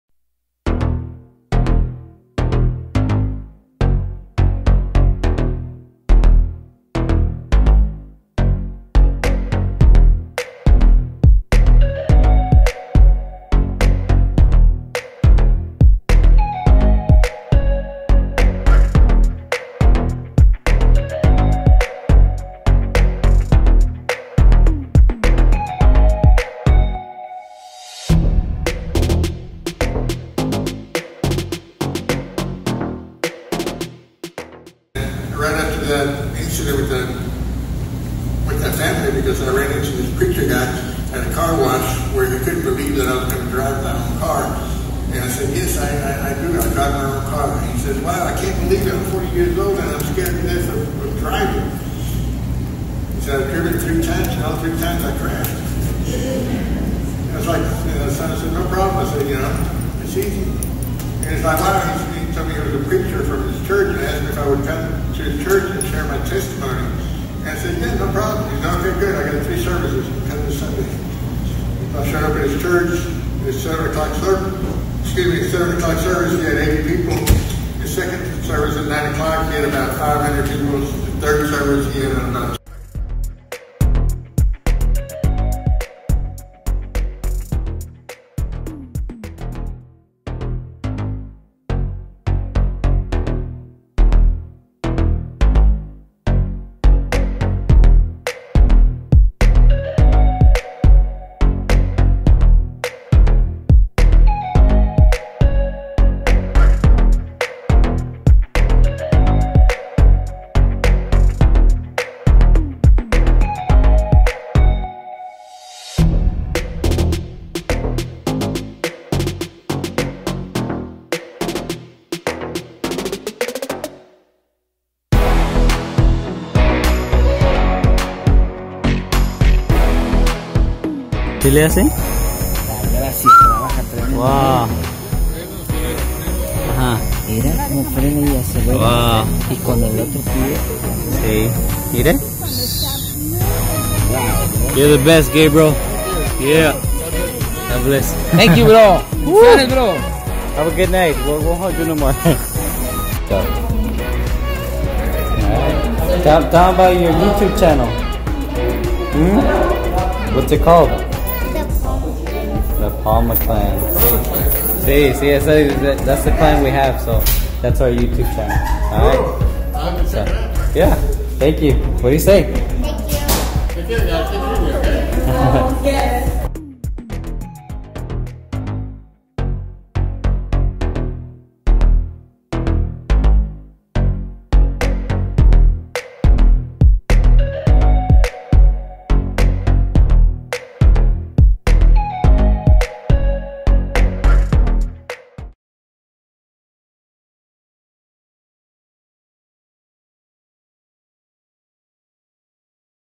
Watch this and you'll never COMPLAIN about anything again in your life! ThePalmaClanJZS
Hello Palma Clan we just wanted to give ya'll some motivation that we received ourselves from this awesome person. Meet Gabriel Najera, he is a great motivational speaker, he was born without arms and his lower body didn't develop like it should. Nevertheless, that didn't stop him from being successful and doing things that we only wish we could! Help us make the Palma clan bigger by joining!